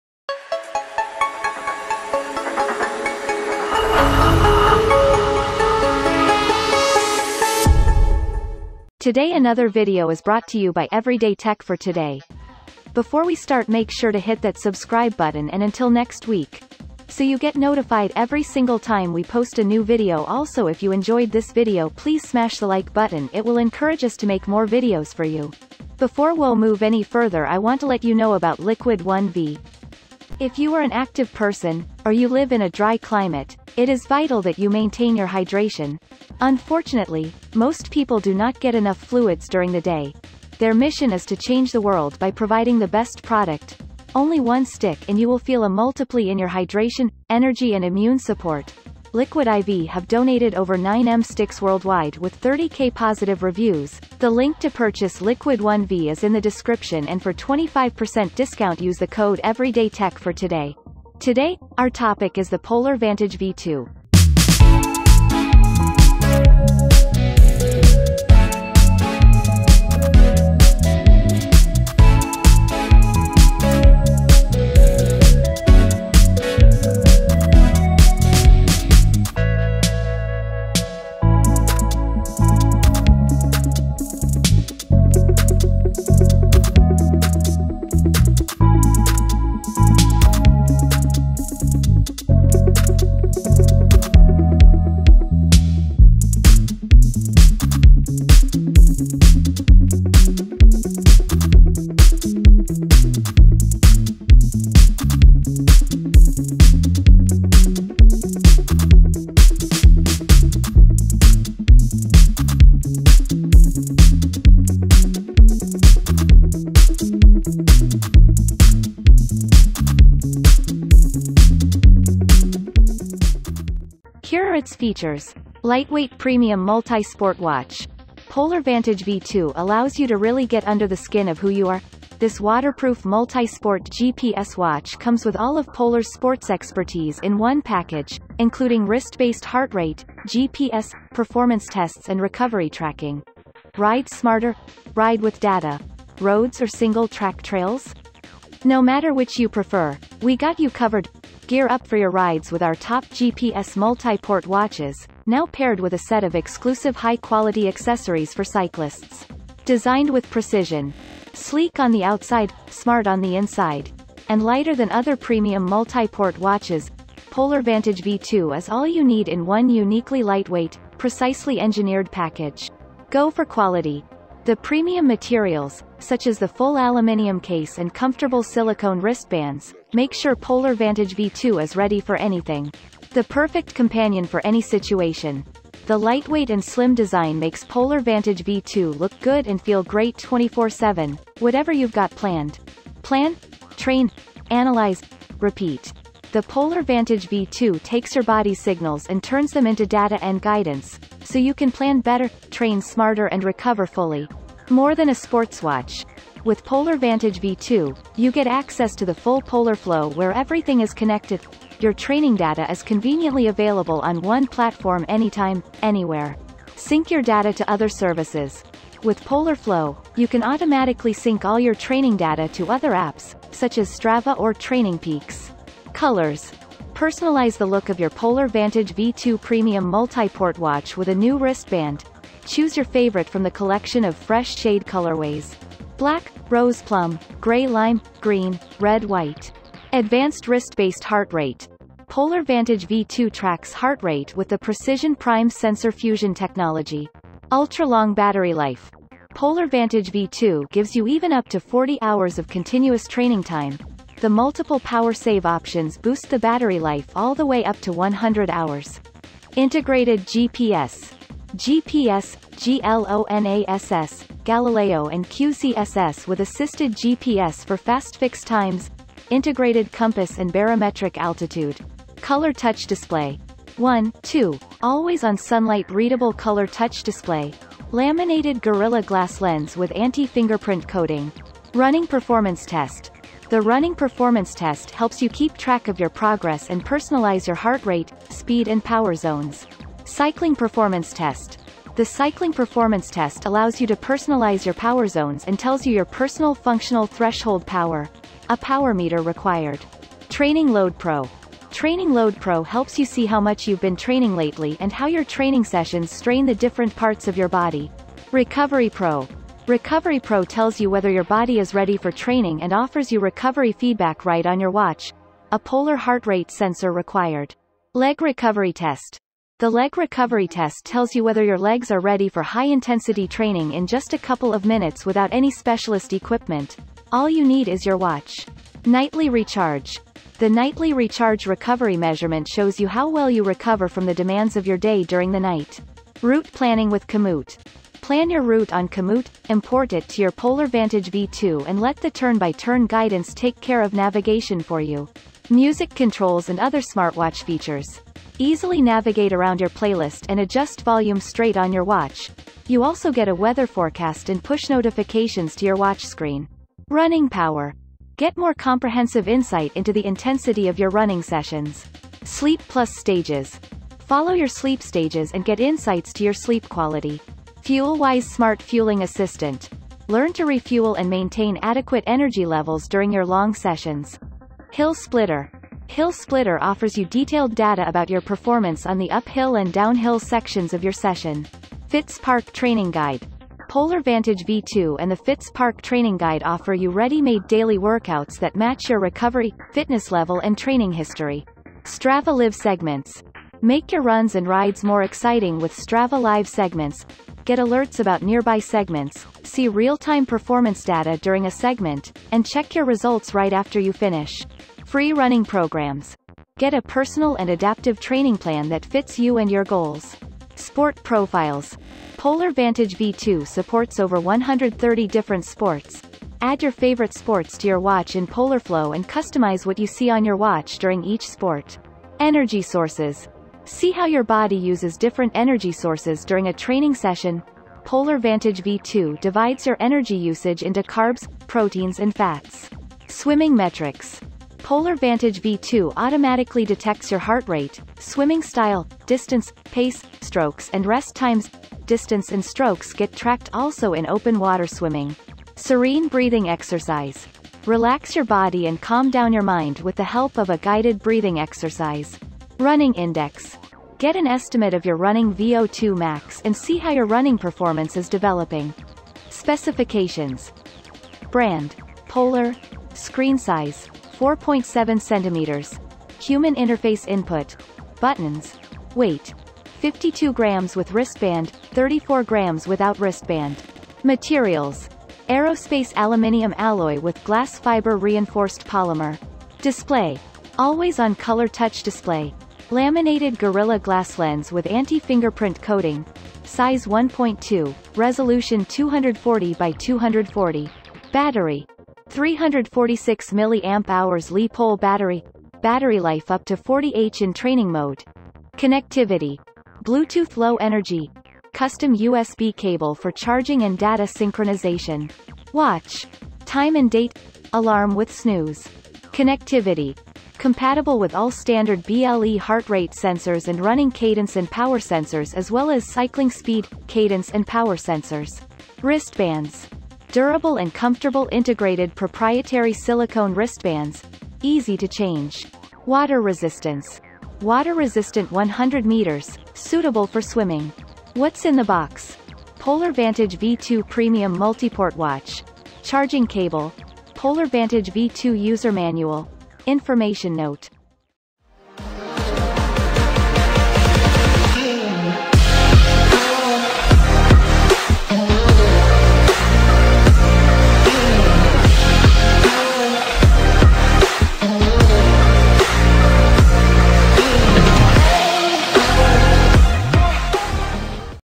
Polar Vantage V2
- The lightweight multisport smartwatch with full aluminum body (only 52g), equipped with advanced wrist-based HR tracking, GPS, ultra-long battery life, running & cycling performance tests, FuelWise, route guidance, sleep tracking, & more.
- The training load and recovery tracking tools help you optimize your training and avoid overtraining. Test your performance and personalize your heart rate, speed, and power zones with the running and cycling performance tests.
- Use Polar Vantage V2 with Polar Flow - a free online tool for planning and following up on your workouts, analyzing activity progress, and sleep. Available on desktop and mobile.
- Connect easily with online sports communities for road running, swimming, cycling, cardio, triathlon (MyFitnessPal, Endomondo, Nike+ Run Club, Strava, TrainingPeaks, Adidas Running by Runtastic, etc.)
- Polar Vantage V2 comes with the essential smartwatch features: music controls, weather, and phone notifications.

➤Liquid I.V.